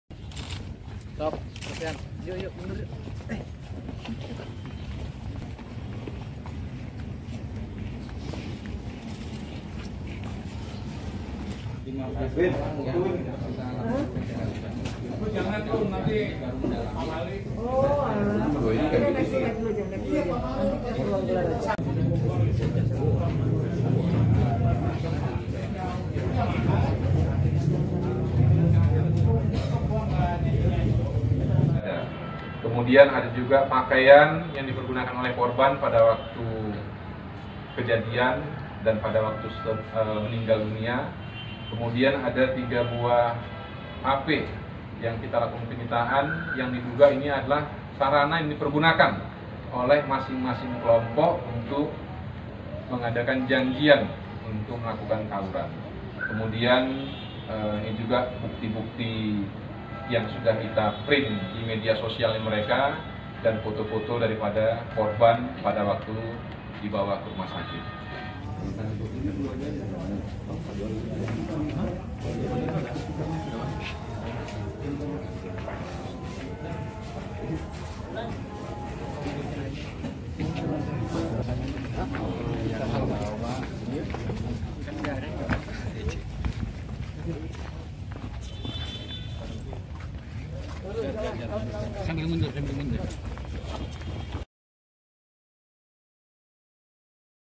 Tawuran Anak Belasan Tahun, Satu Orang Tewas
Polresta Bogor Kota Berhasil meringkus para pelaku di bawah umur yang melakukan aksi tawuran di Jalan Roda Babakan Pasar, Kota Bogor. Yang menyebabkan satu orang remaja lainnya tewas berinisial F (18), pada Sabtu 17 September 2022 dini hari lalu.

Para pelaku yang masi berusia belasan tahun tersebut berjumlah lebih dari lima orang. Dari tangan pelaku, petugas berhasil menyita barang bukti berupa senjata tajam jenis cerulit.